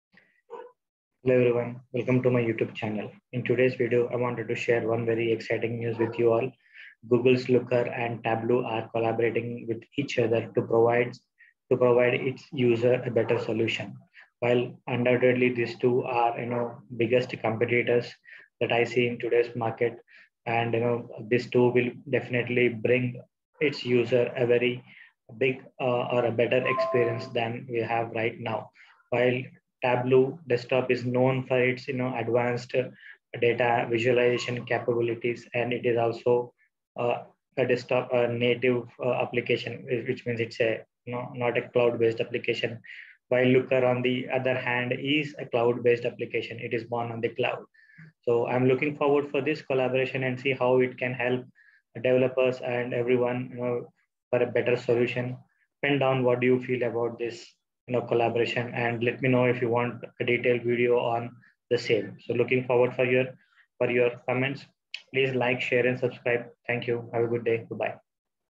Looker and Tableau Partnership
Hello Everyone,

Tableau and Google Cloud are partnering to help modernize your data and analytics infrastructure and unlock your data’s full value. Google's Cloud Platform is the enterprise solution of choice for many organizations with large and complex data problems. With Tableau, any user can visually explore that data in real time. With no need to move data to in-memory storage, you can connect to and analyze data wherever it lives, taking full advantage of Google Cloud’s computing capacity—and providing an end-to-end analytics solution.